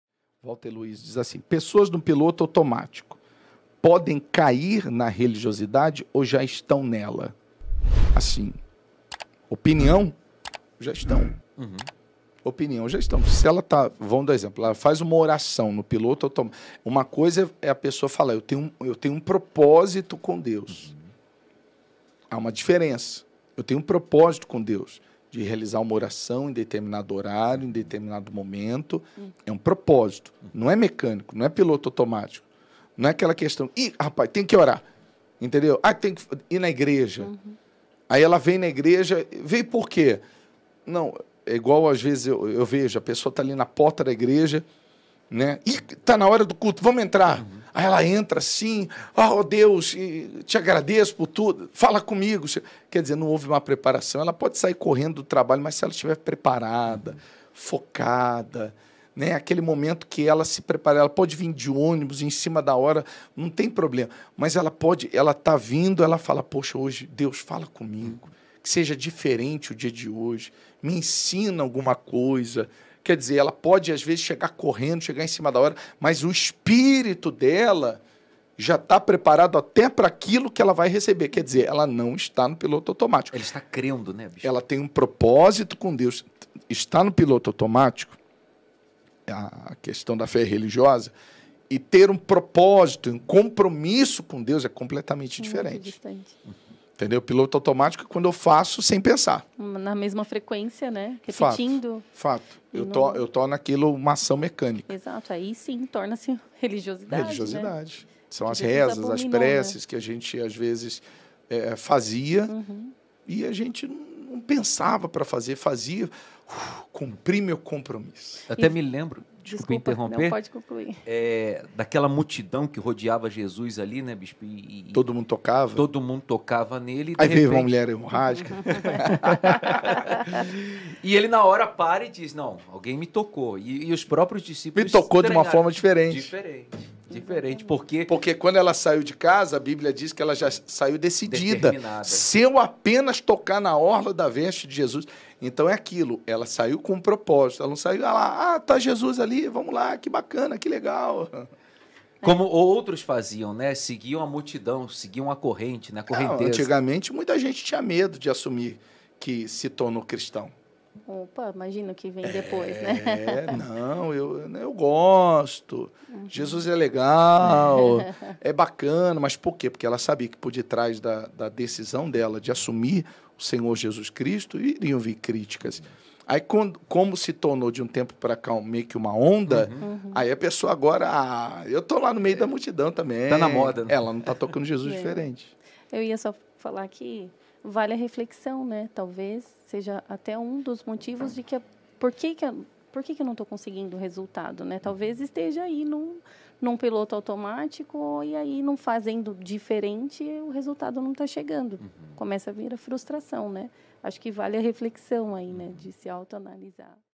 Ter propósito com Deus ou religiosidade: o que faz sentido na fé? | Cortes Podlive
Os dias são tão corridos que facilmente nos deixamos cair na armadilha do piloto automático.

Fazemos muito mas, sem pensar, sem planejar, sem prévio preparo.

Seja nos afazeres domésticos, do trabalho e até mesmo nas nossas relações pessoais 😦

Por que?
Porque já sabemos como fazer!

E no seu relacionamento com Deus:
- Você cumpre rituais por fazer?
- Você tem um propósito com Deus?
- Você ora? Por que?
-Você repete rituais religiosos sem saber o real motivo?
- Você é um cristão no piloto automático?
- Você ora ou vai à igreja por obrigação?
- Você se relaciona com Deus de forma diferente? Praticando a sua fé ou só repete, repete, repete e acaba não chegando a lugar nenhum?

- Você pratica religiosidade ou fé inteligente?

Quem é você?